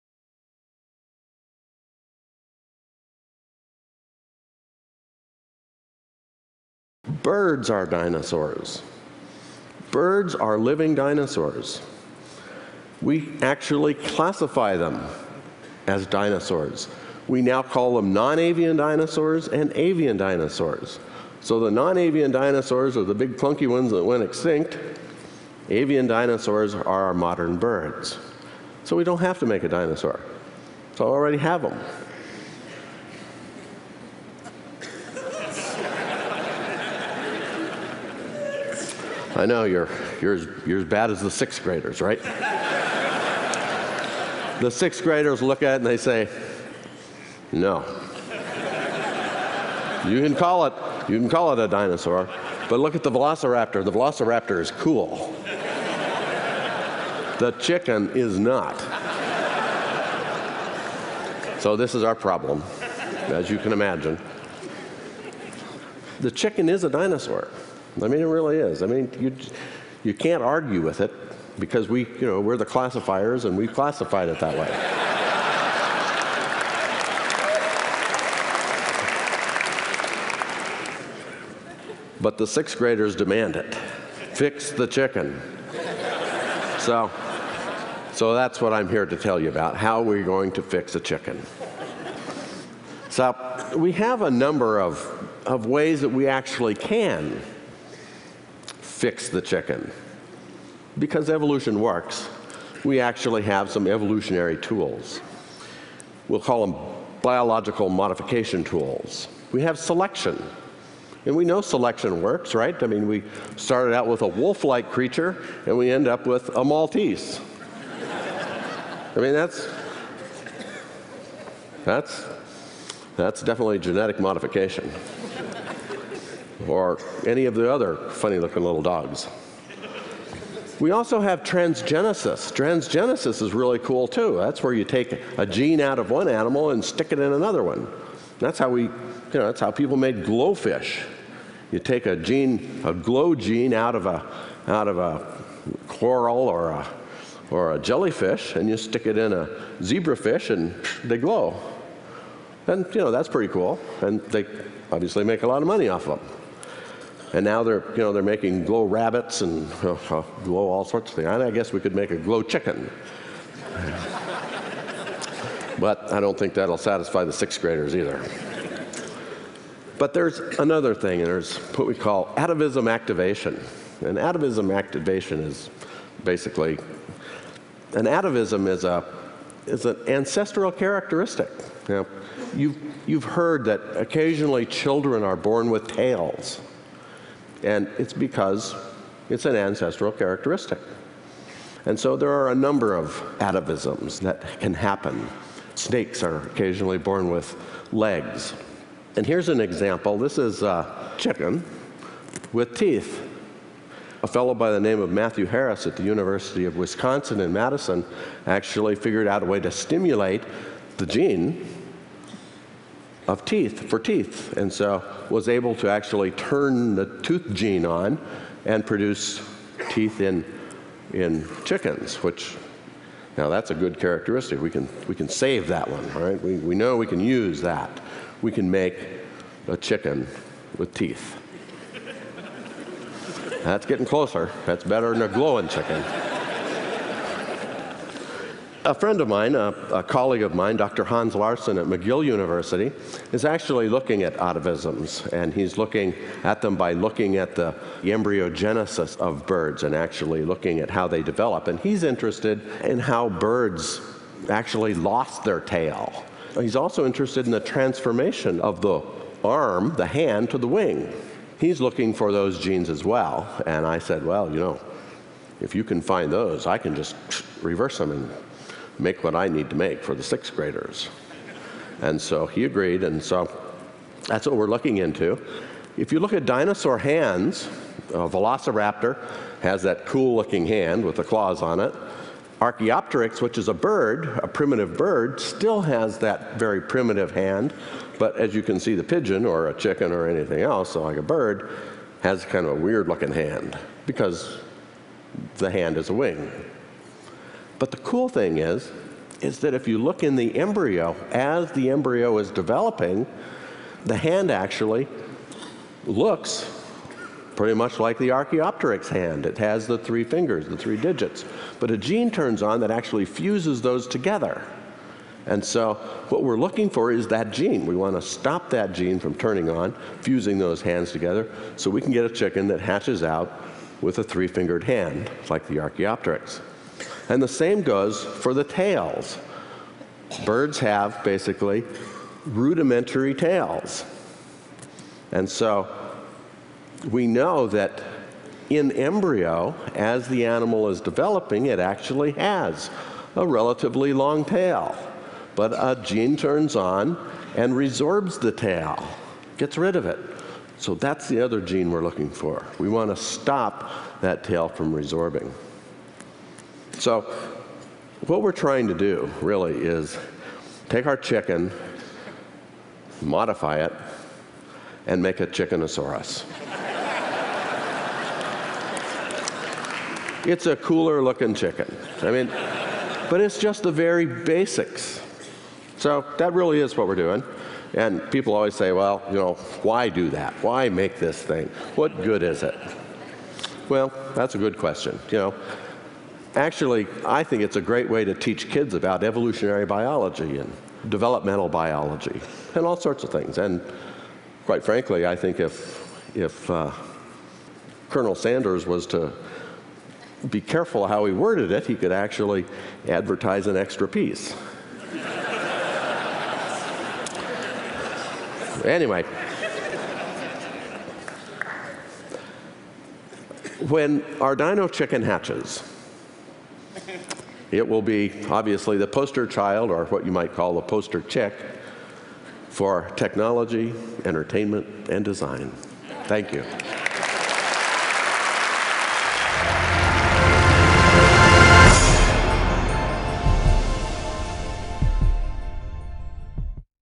Transformando Uma Galinha em Dinossauro
O paleontólogo norte-americano Jack Horner diz que seu sonho desde criança era ter um dinossauro. De forma bem-humorada ele relata que primeiramente procurou nos fósseis de dinossauros alguma amostra de DNA intacto (Jurassic Park), mas após várias tentativas frustradas ele teve a ideia de utilizar os parentes vivos mais próximos dos dinossauros, as galinhas. Procurando induzir um processo de "involução" Jack e outros pesquisadores estão reativando os genes que foram deixados de lado ao longo do processo evolutivo das galinhas. O objetivo final da equipe é chegar o mais próximo possível de um Frangossauro.